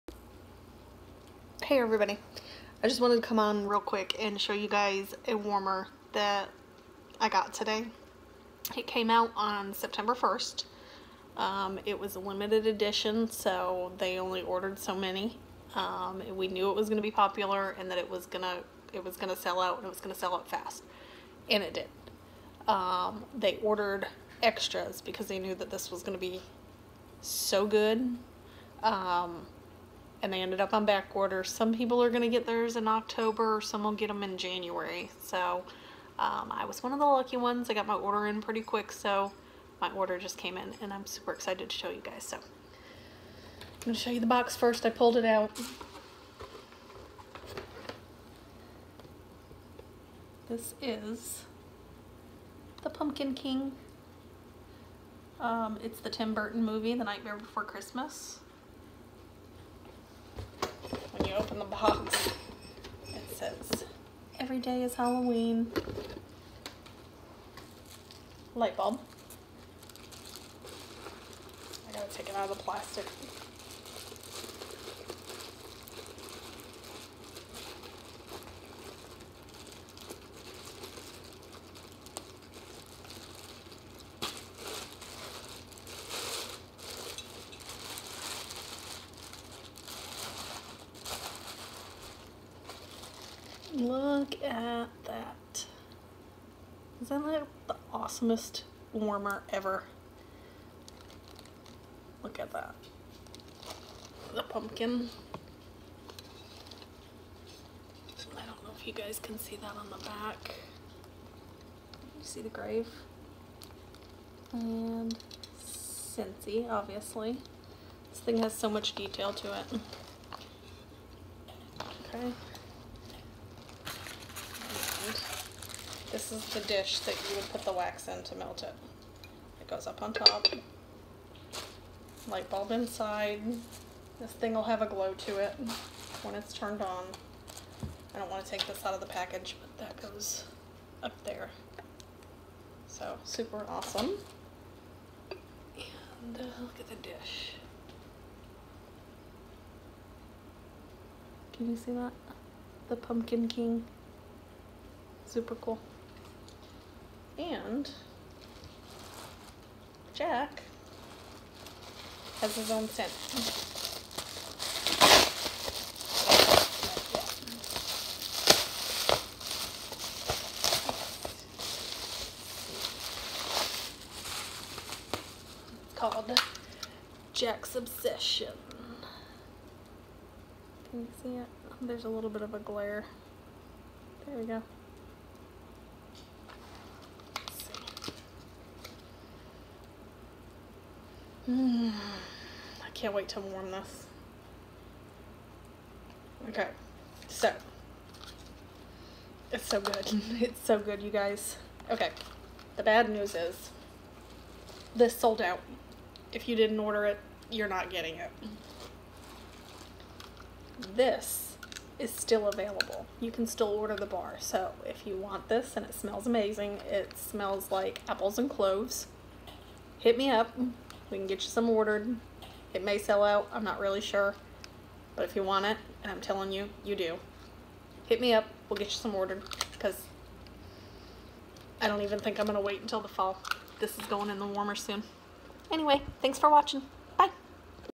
Jack Skellington Pumpkin King
Our Jack Skellington Pumpkin King Scentsy warmer is absolutely the best! And I literally can’t begin to tell you how amazing the Jack’s Obsession Scentsy wax bar is. It’s a cauldron full of apple spiced with clove and cinnamon that makes for a ghoulishly grand brew.